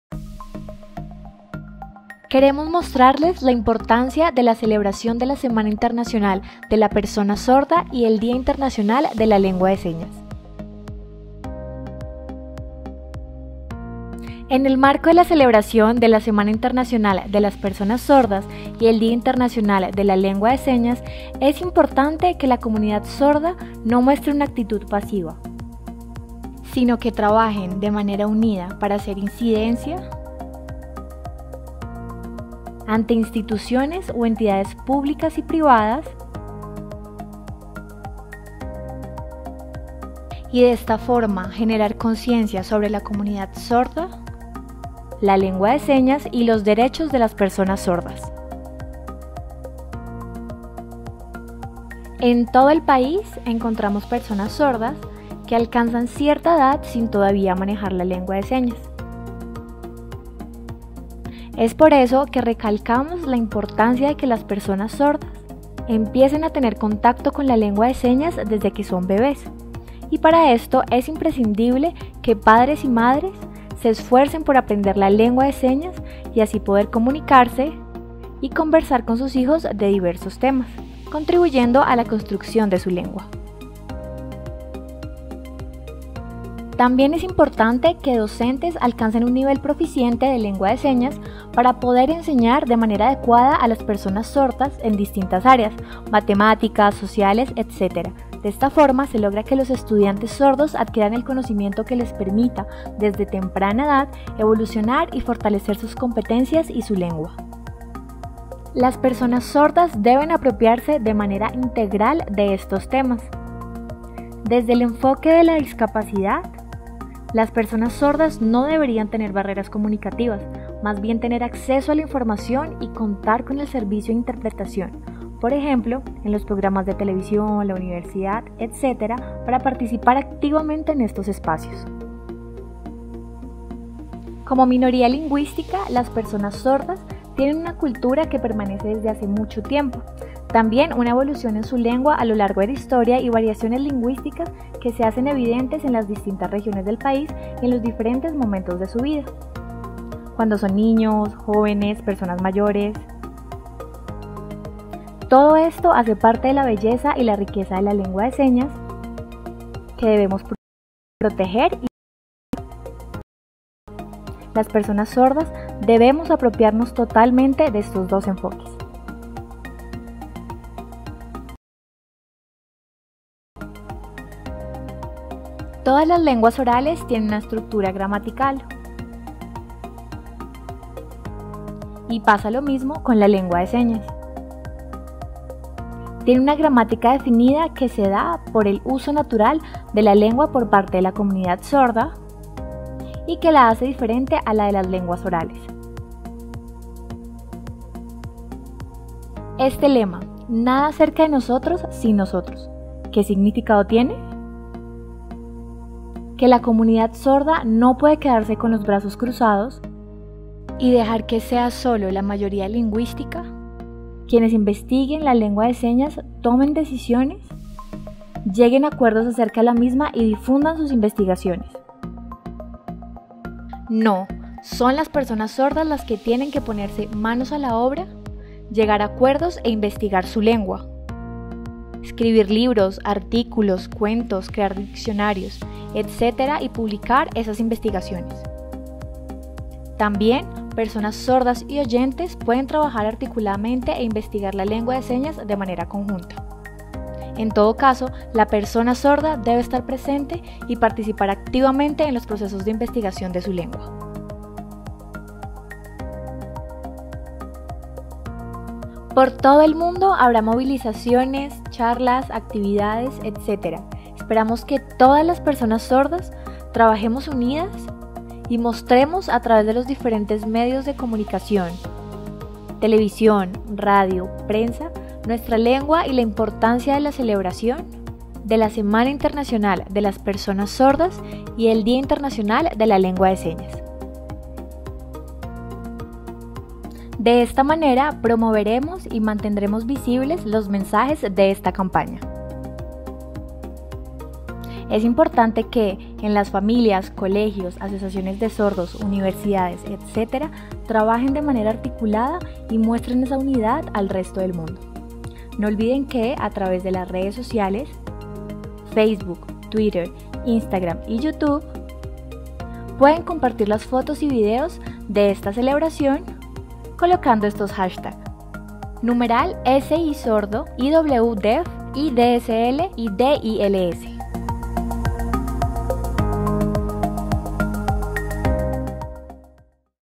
Semana Internacional del Sordo
¡Estamos en la Semana Internacional del Sordo y celebramos por primera vez el Día Internacional de la Lengua de Señas!. Apropiarnos de estos temas es fundamental para que no existan barreras en la comunicación y así se fomente la participación activa de la comunidad sorda en diferentes espacios.